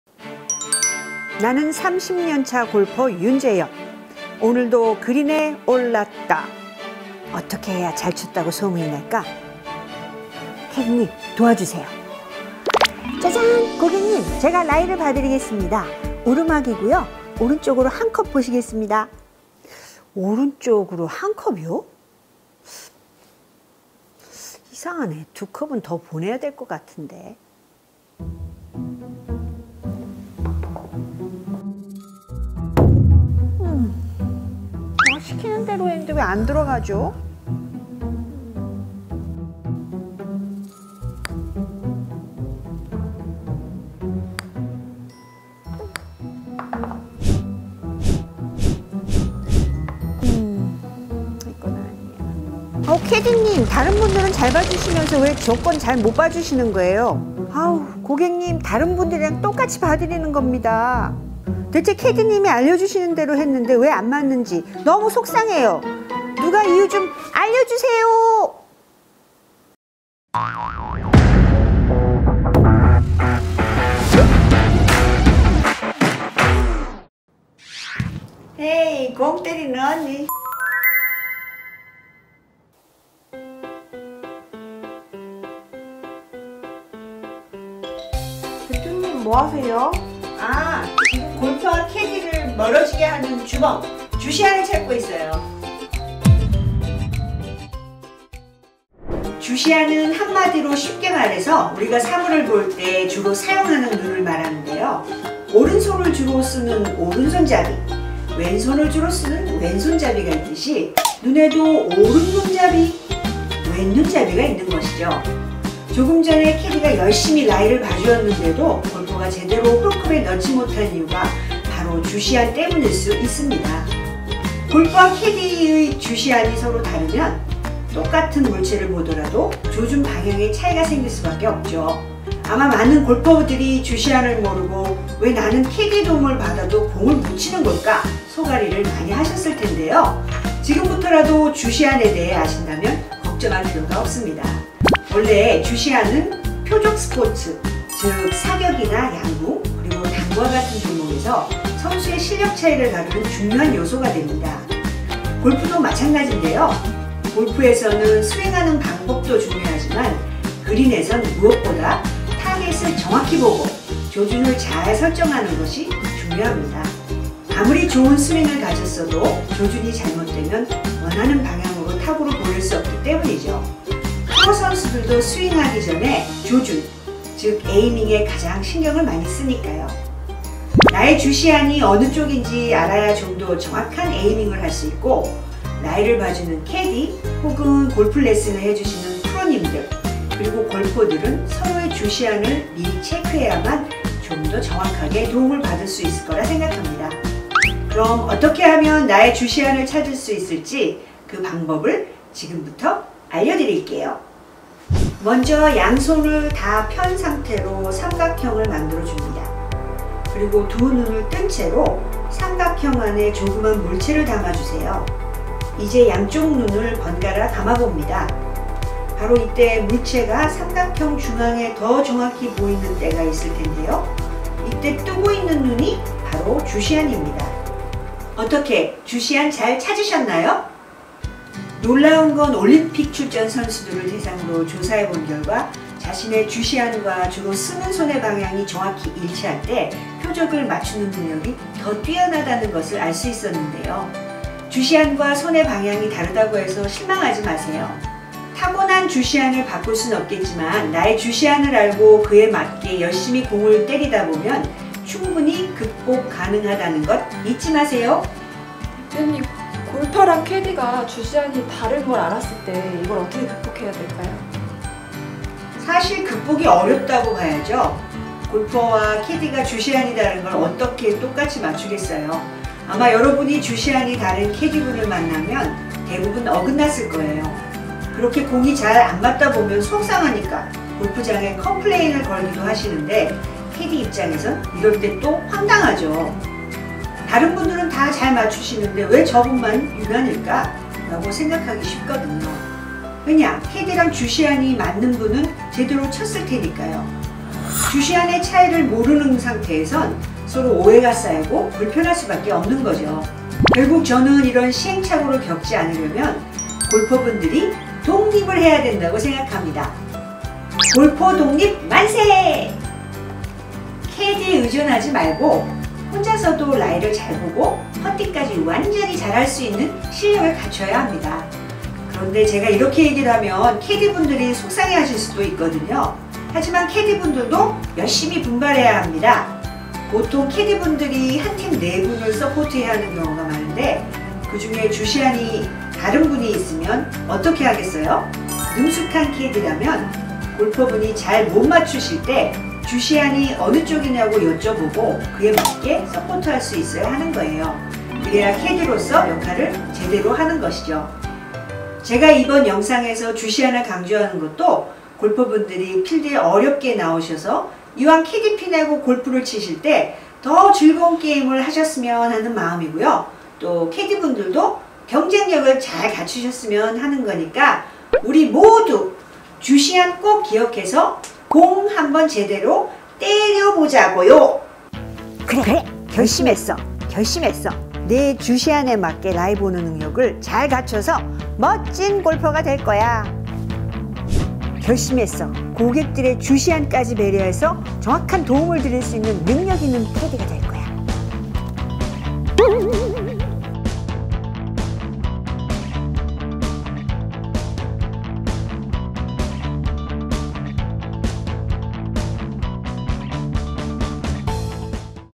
골프칠 때 유독 나랑 안맞는 캐디, 원인은 주시안! -주시안의 비밀-
홀컵 앞에서 자꾸만 작아지는 골퍼들!
캐디의 도움까지 받아봐도 왜 내 공만 자꾸만 빗나가는 걸까?
알고보면 분명한 이유가 있다는데... - 더보기-

안녕하세요, 공 때리는 채널 구독자 여러분!

'골린이 머리올리기 프로젝트'를 마무리 하고
다시 새로운 영상으로 찾아왔습니다

혹시 여러분 '주시안'이라고 들어보셨나요?

당구와 농구, 사격 그리고 골프까지
표적 스포츠를 잘 하기 위해서
꼭 알아야 할 필수 상식인데요

특히 골퍼분들 중에
"캐디의 도움을 받아서 에이밍을 해도, 나만 매번 빗나간다!"
이렇게 속앓이를 하셨던 분들!
이번 영상 놓치지 마세요!

캐디와의 간극을 좁히고
퍼팅 실력을 쭉쭉 늘릴 수 있는 비법을
공 때리는 언니가 전수해드립니다

그리고 이번 영상에서 놓쳐선 안될 한 가지!
1인 2역 연기에 도전한
공 때리는 언니의 활약도 기대해주세요!

 [영상개요]

 - (상황극) 골퍼와 캐디의 흔한 갈등! 00:55
 - 골퍼와 캐디의 갈등 원인은 무엇? 01:16
 - 주시안이 대체 뭐길래? 01:43
 - 주시안 찾는 방법 전격공개 03:26
 - (돌발질문)
  골퍼와 캐디! 주시안이 다를 때 극복할 수 있는 방법 05:15